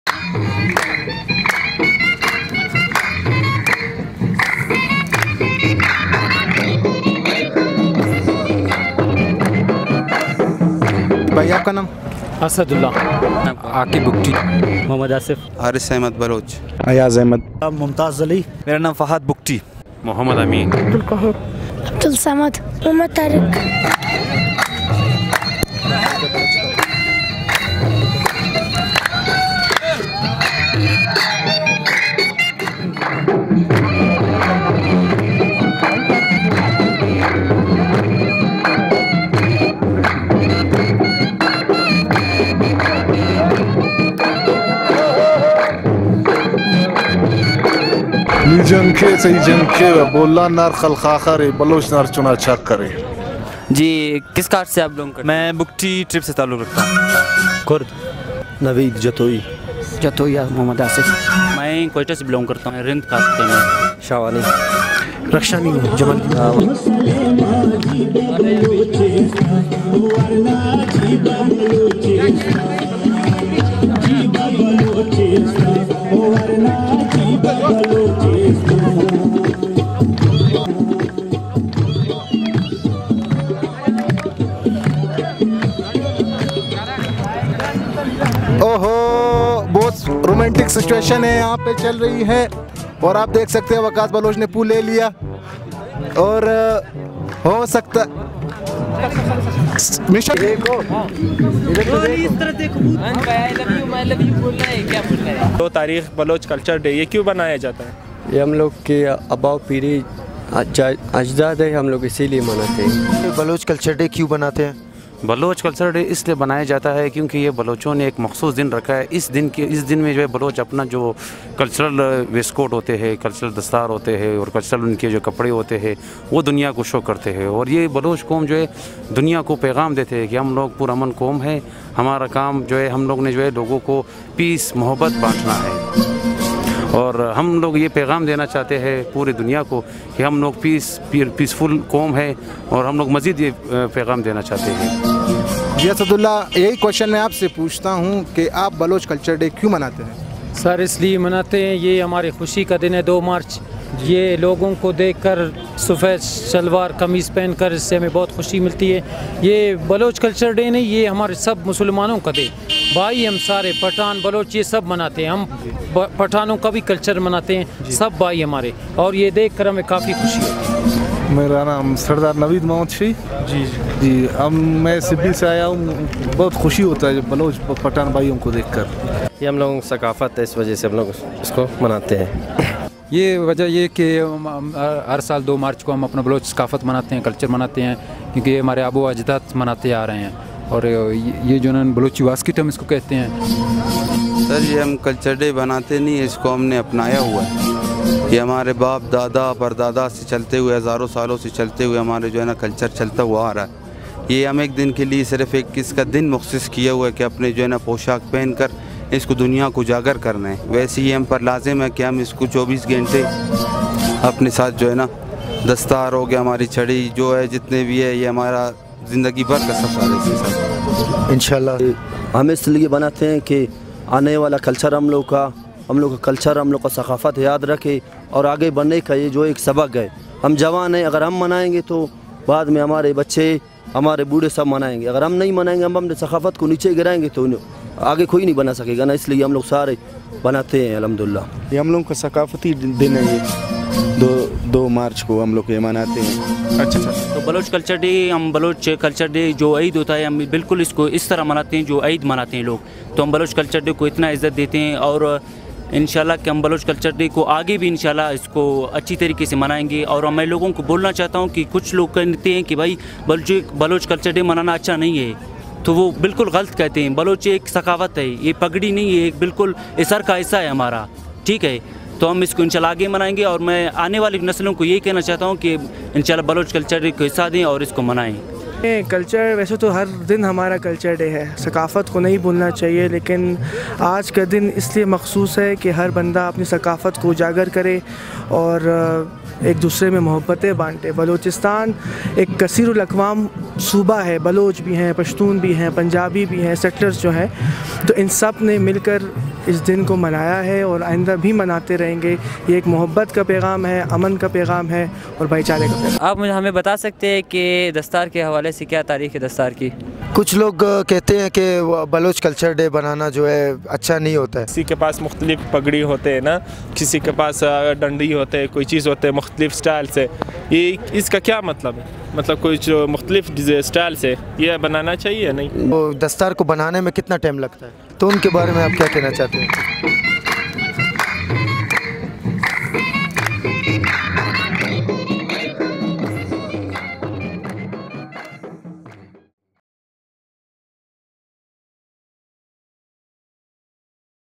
2 March Baloch Culture Days 2022 Part 1
Part 1 and part 2 coming soon
Interview was University of Balochistan Quetta
1.Waqas Baloch
2.Hamad Ali Shah Yousafzai
3.Abdul Waheed
4.Ahmad Khan